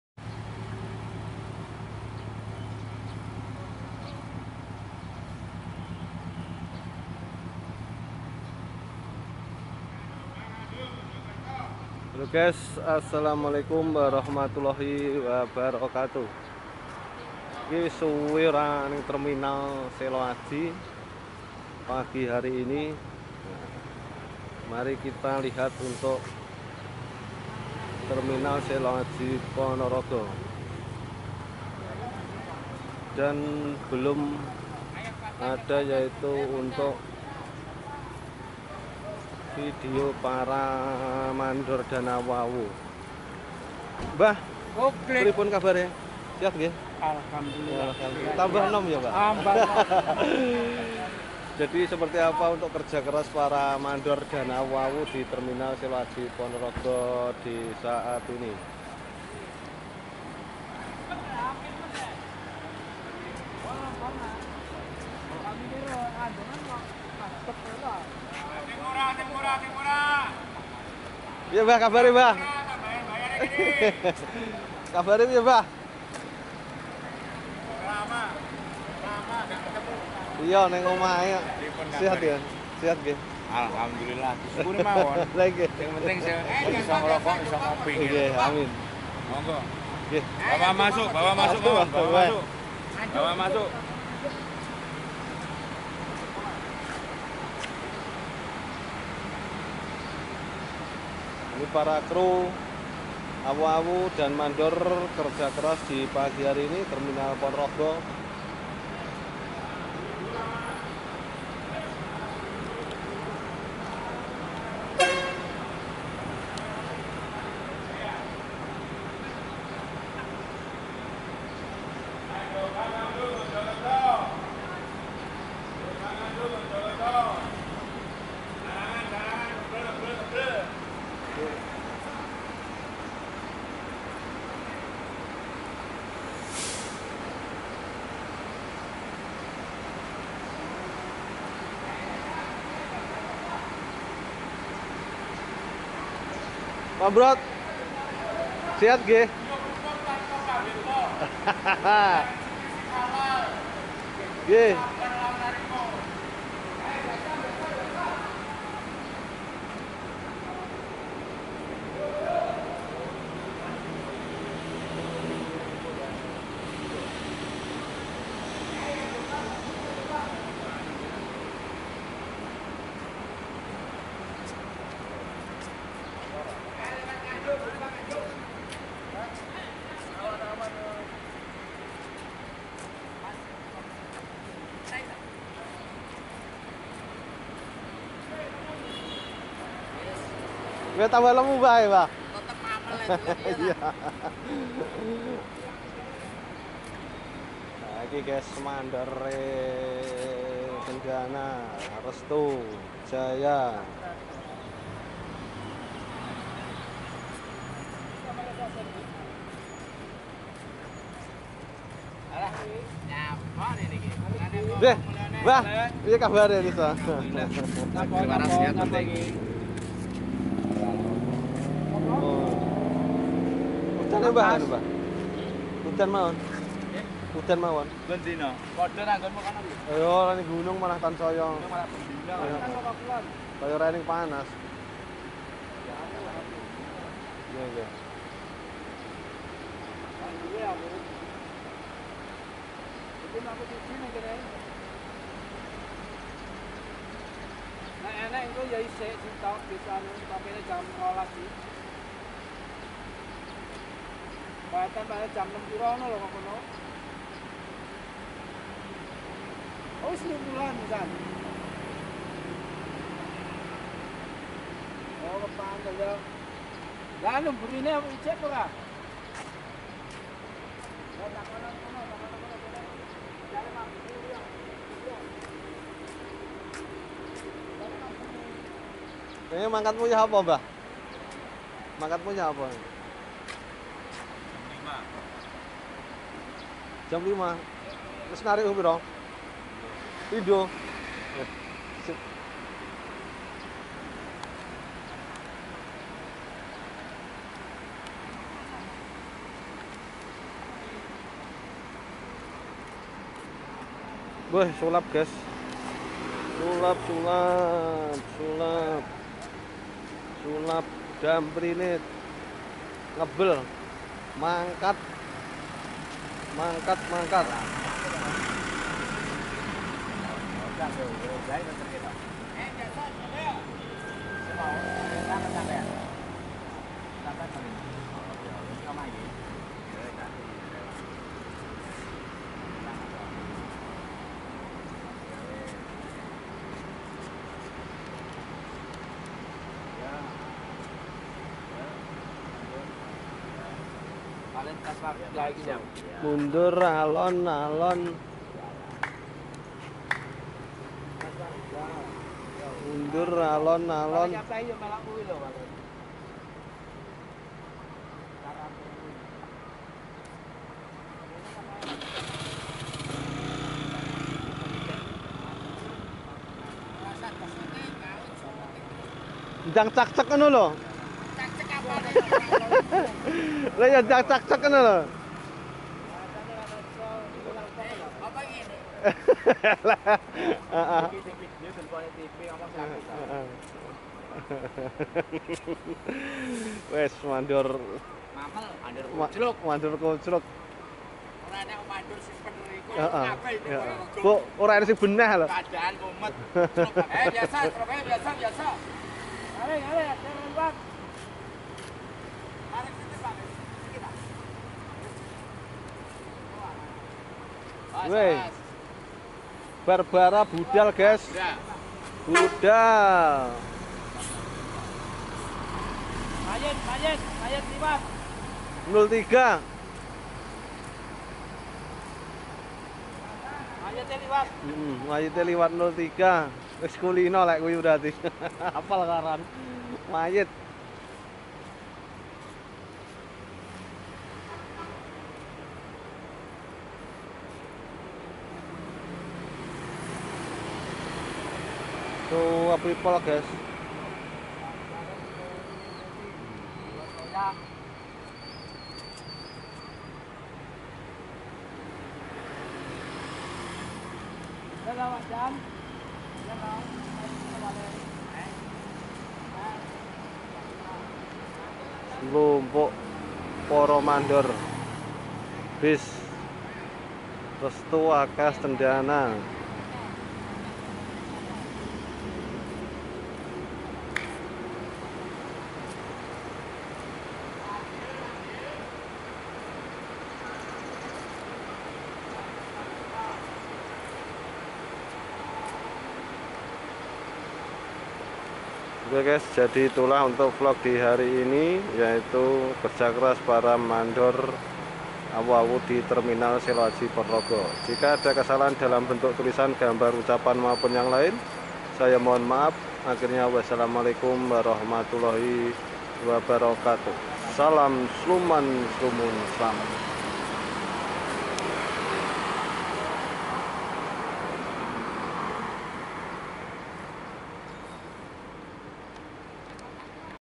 BEGINILAH KERJA KERAS PARA MANDOR,CREW DAN AWU AWU BUS DI TERMINAL SELOAJI PONOROGO
Masih dari kawasan seputar Terminal Tipe A Seloaji Ponorogo vlog dan hunting bus.Kondisi dan kerja keras para crew,mandor dan AWU AWU bus untuk cari penumpang di Terminal Tipe A Seloaji Ponorogo saat ini.